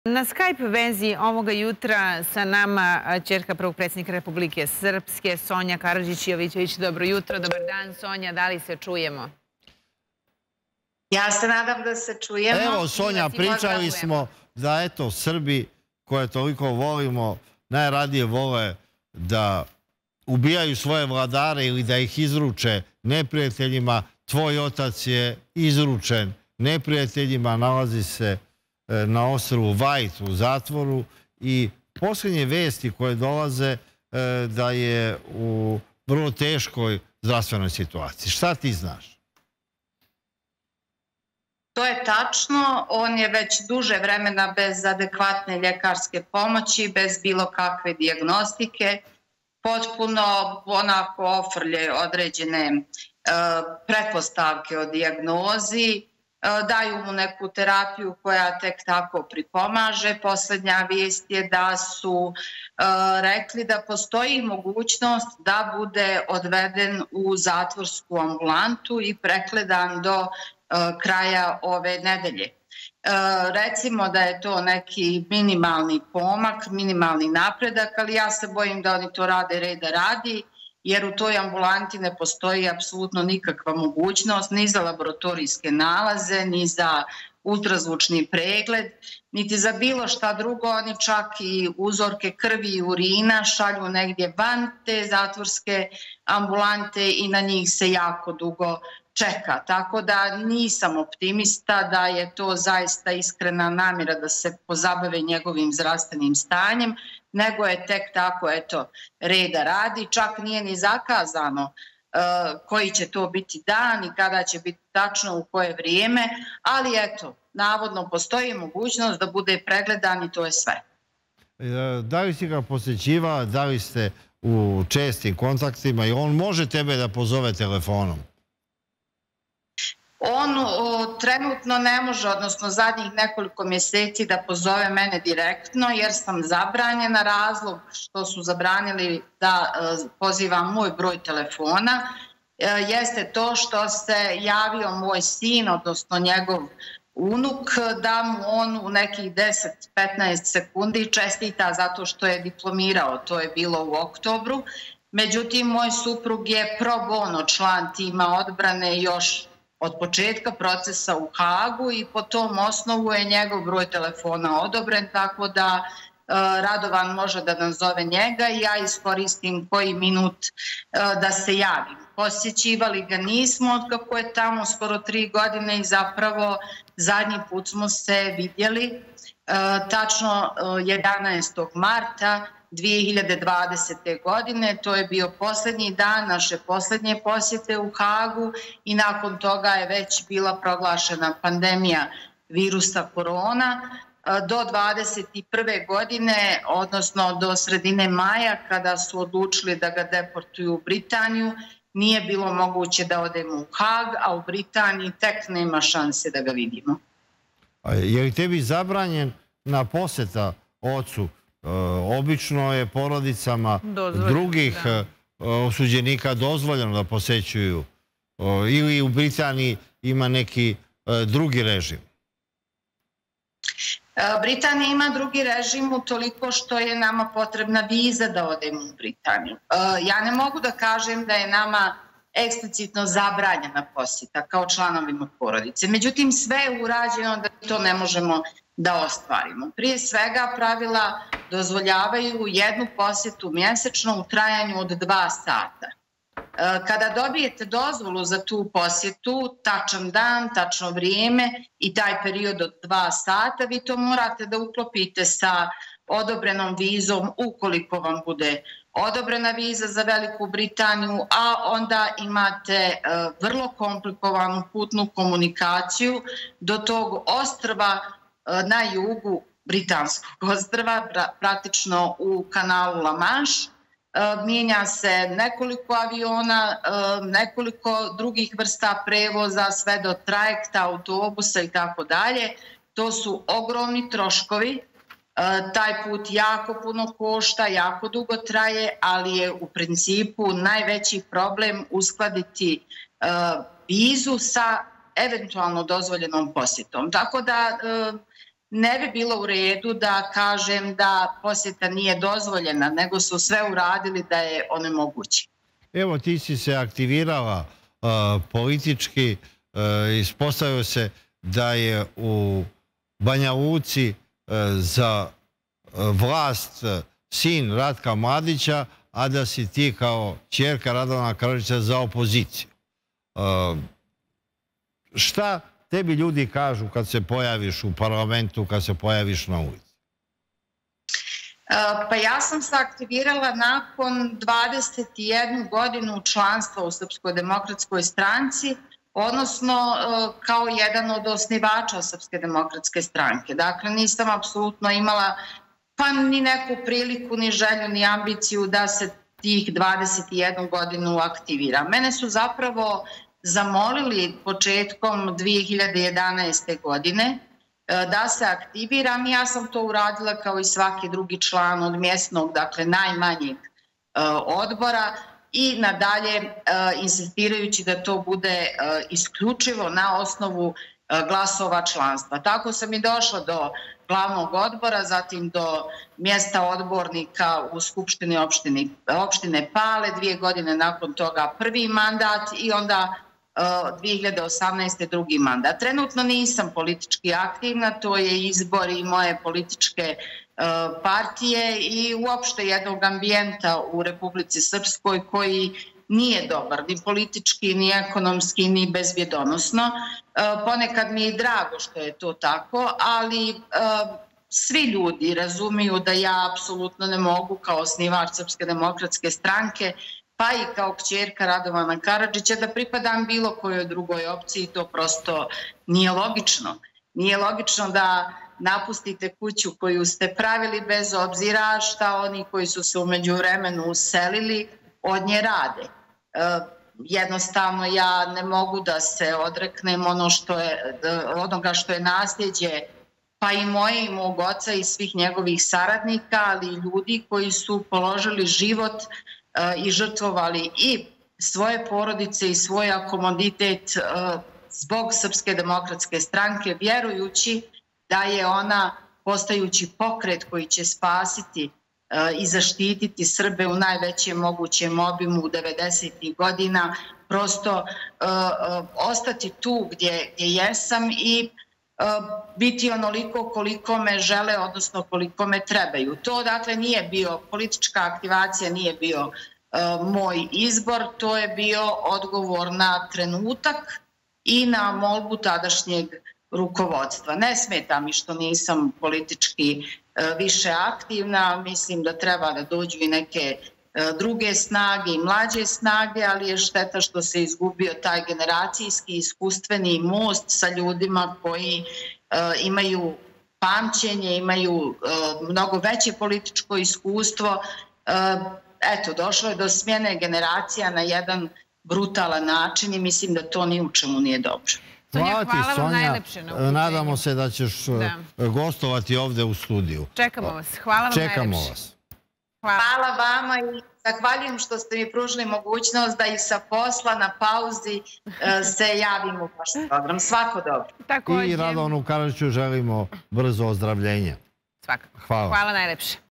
EKSKLUZIVNO -Sonja Karadzic, cerka Radovana Karadzica- "Sve je uradjeno da ne mozemo da ga posetimo"
Uključenje: Sonja Karadžić-Jovičević - Ćerka Radovana Karadžića